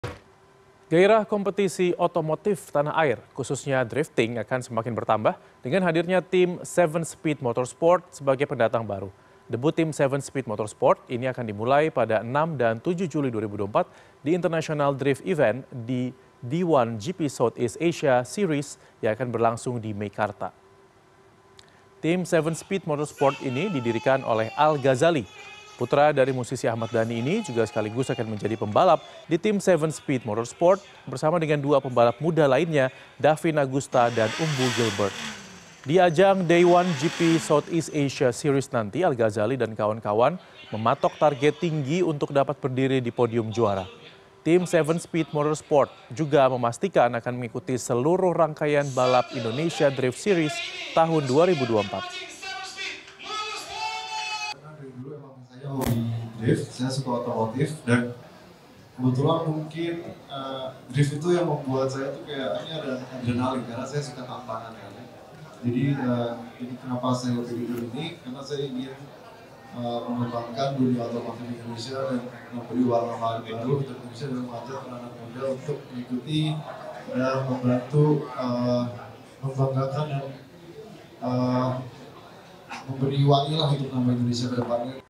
Al Ghazali Bentuk Tim Balap Drift
Al Ghazali, putra musisi Ahmad Dhani membentuk tim balap drift  dengan nama tim Seven Speed Motor Sport.